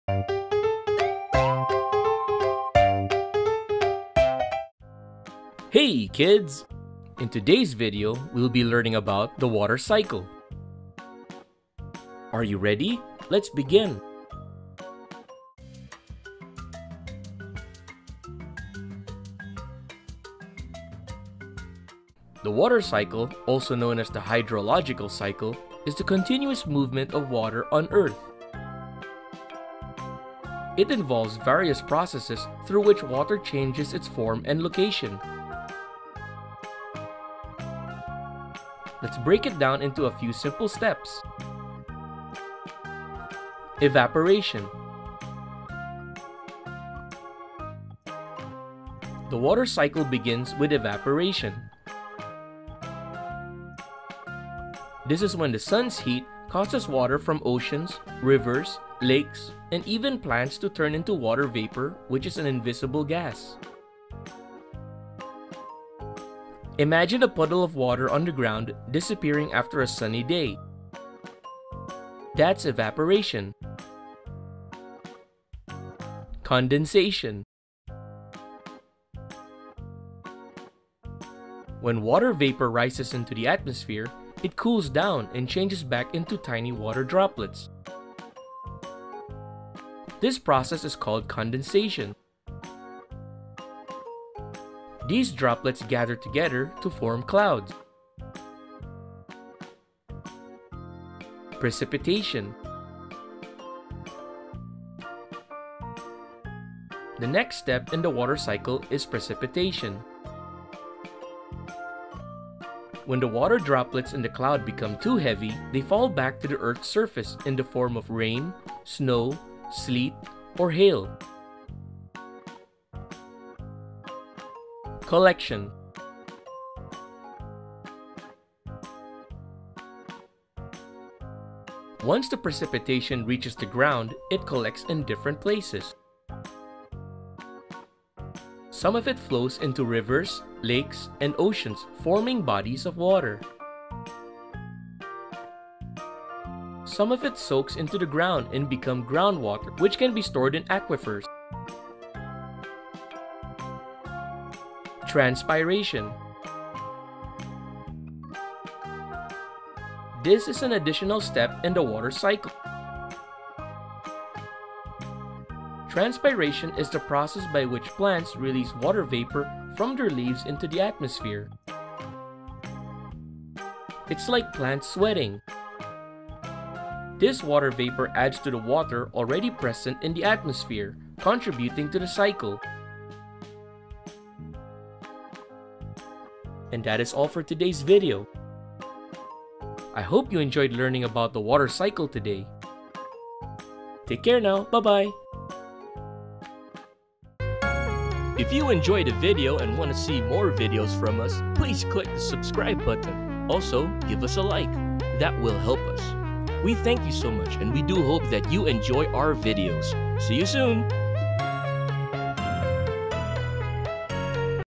What is the Water Cycle | Science for Kids
Hey kids!

In today's video, we will be learning about Water Cycle.

Did you know that the water cycle is also known as the hydrological cycle?
0:09 The water cycle
0:41 water cycle process
0:44 Evaporation
1:15 Condensation
1:41 Precipitation
2:04 Collection
2:35 Transpiration

If you want to know more about Water Cycle, come on in and join us in todays lesson.